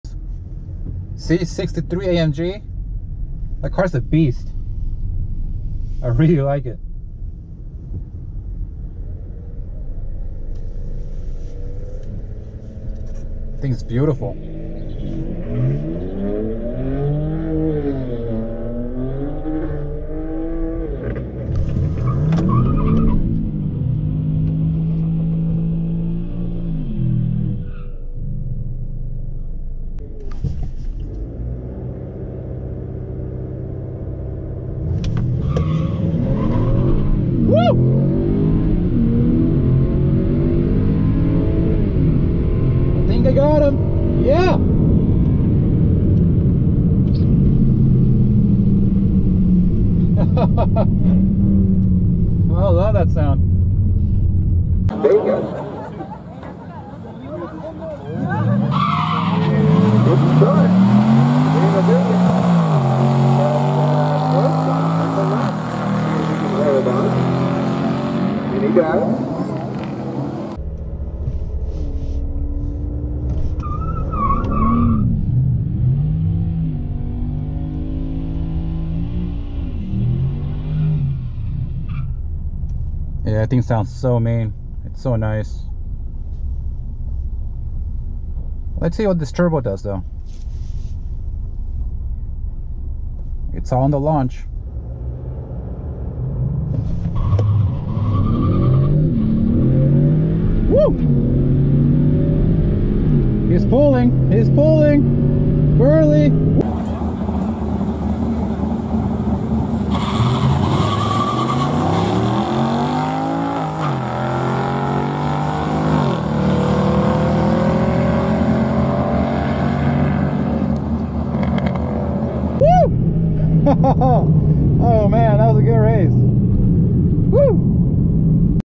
2020 Honda Accord Sport 2.0T vs. 2010 Mercedes C63 AMG: 2 Races
Barona Street Legal Drags 10/23/2020